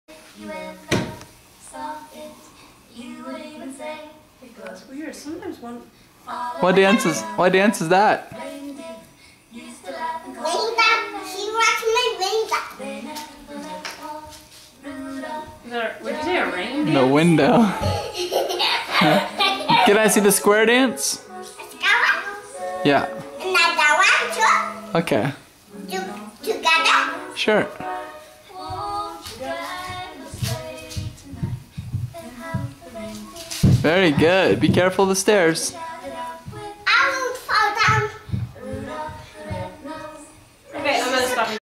11.24.10 Cammy dancing at top of stairs.MPG
Cammy dancing right before bedtime. A short clip of her making up different dance titles, but dancing the same each time.